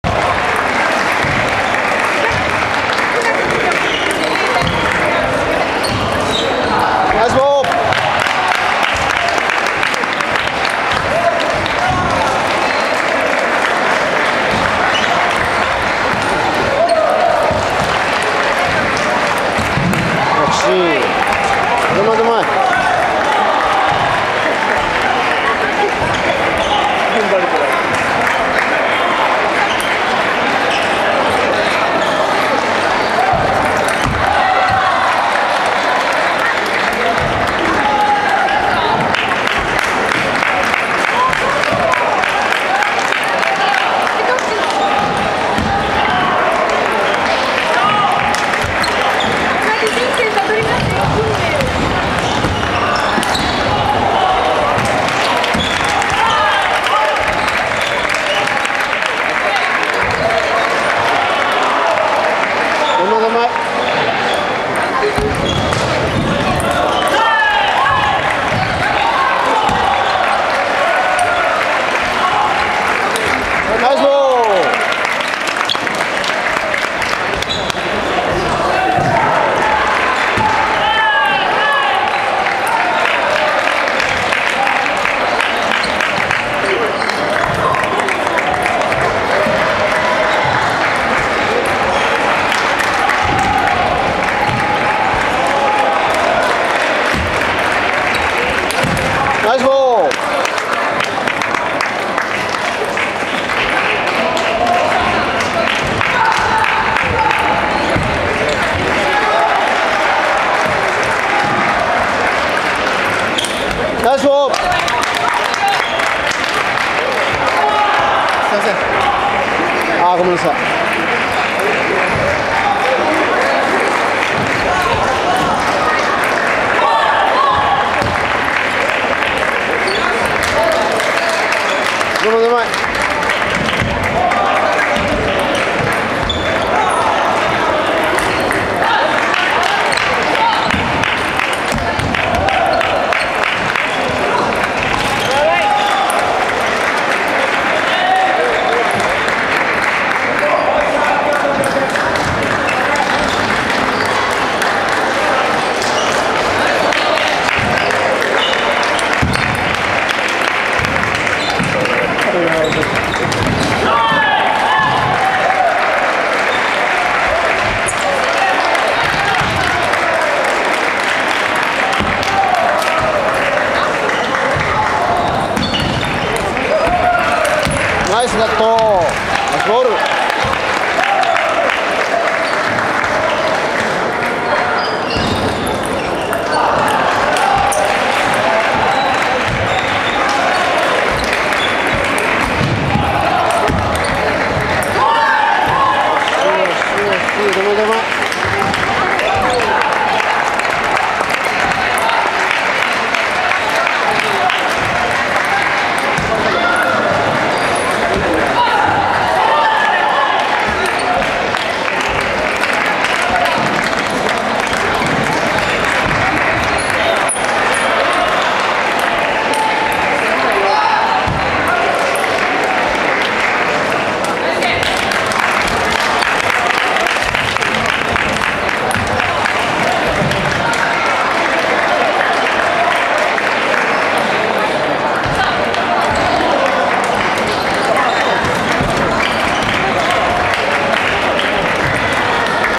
H25年度 第３２回全日本クラブ卓球選手権大会 東京都予選 愛卓TTC パート27
平成25年度の全日本クラブ選手権大会・東京予選の動画になります。
無事２位で予選を通過できました。
出場選手（村守　実・平屋　慶四郎・尾形　貴幸・川口　拡・手塚　理人・金井健太郎）
監督森田　幸二。